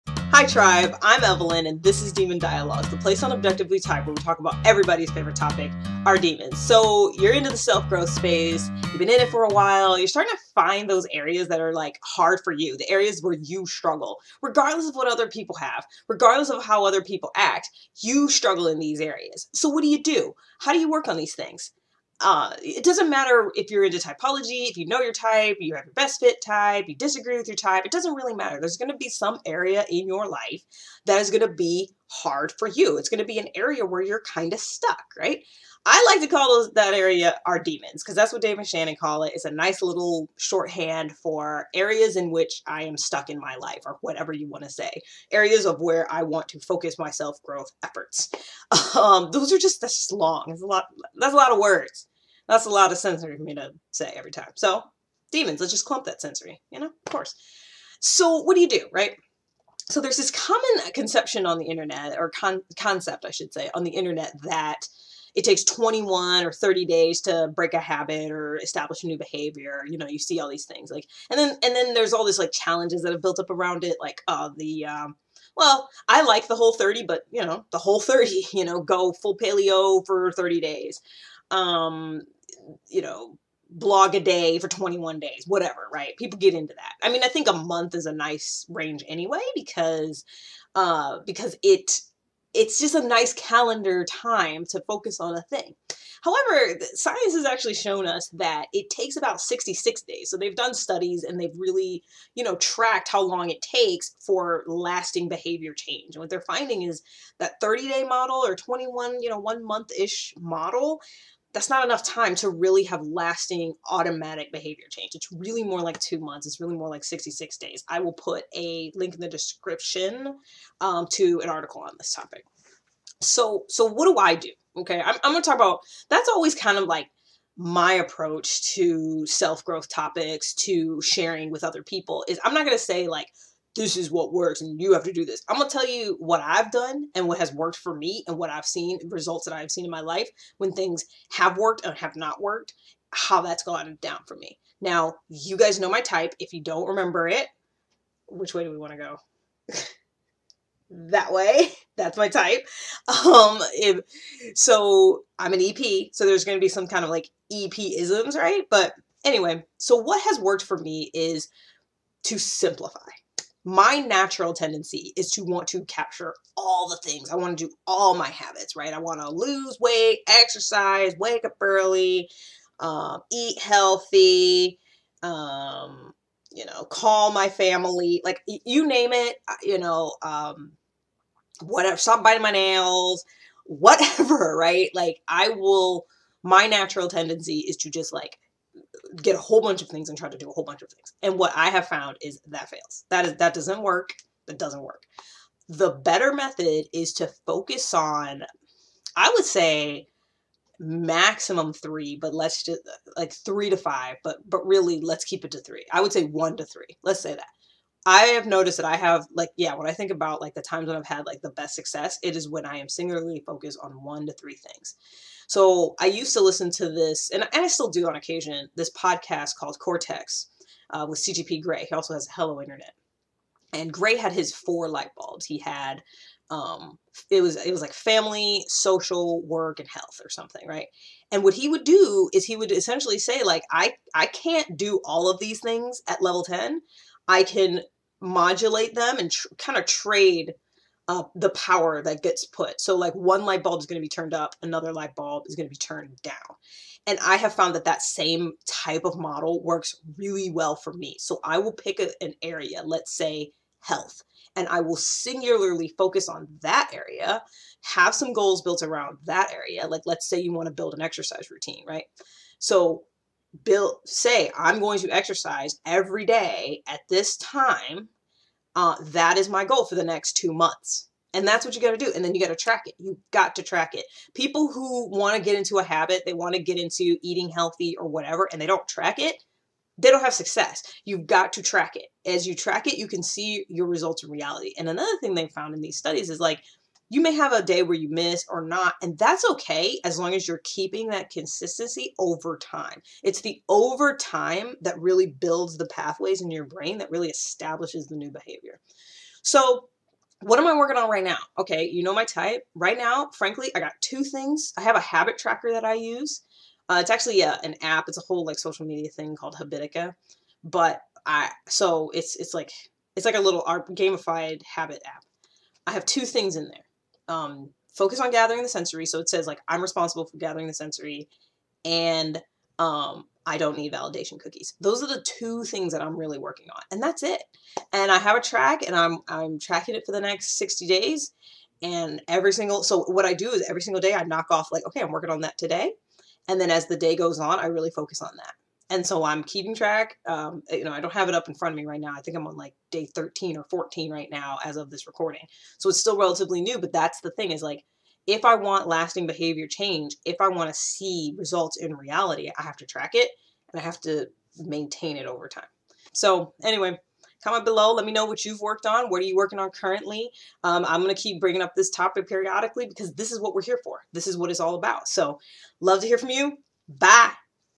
Working on Demons || Demon Dialogue ep 14
In this week's demon dialogues I talk a little bit about what it takes to build a habit and see lasting change.